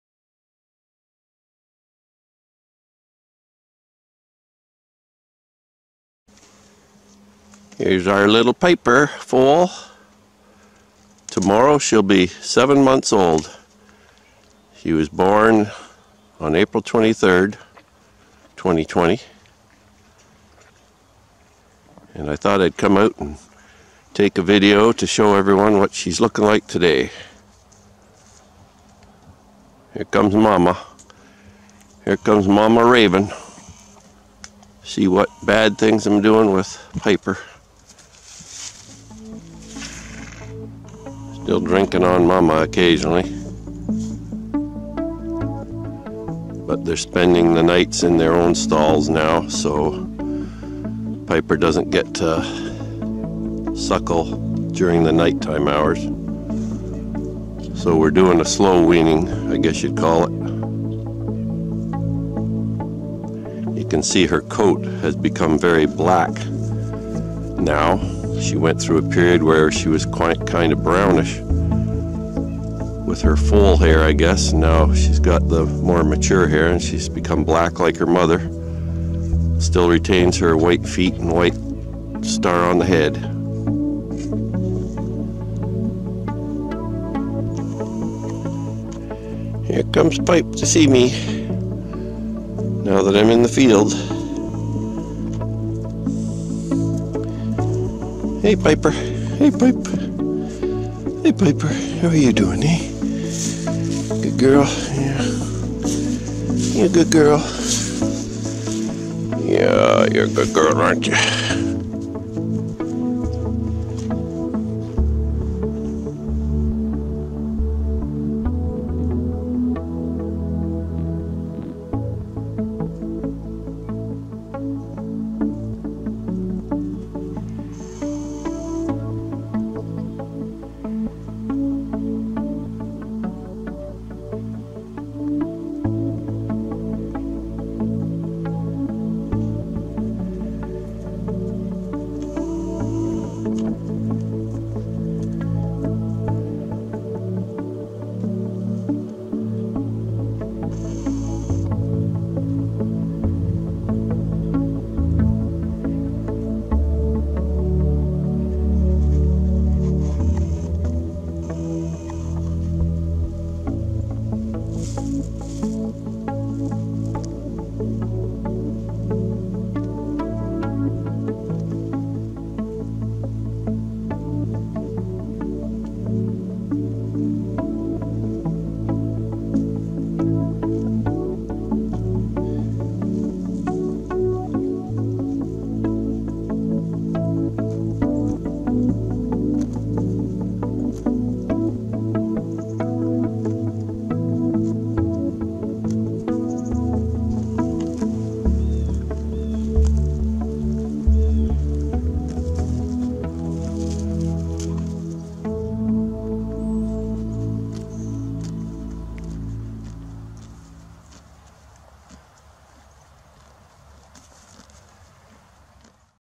Hanoverian Foal at 7 Months Old
Tomorrow, our Hanoverian foal Piper is 7 months old so I wanted to do a short video update on her current appearance.  She was born on April 23, 2020.  She is now about 2/3 full horse size and her coat has become quite black.  She has 3 white socks and a small star on her forehead.  She is out of Raven (mare) X Fabregas (Stallion).

She seems to have lost much of her early playfulness  but hopefully some of that will come back.  It would have helped if we had some other foals for her to play with I guess.  However she still retains a very nice calm and curious attitude which is certainly a big plus.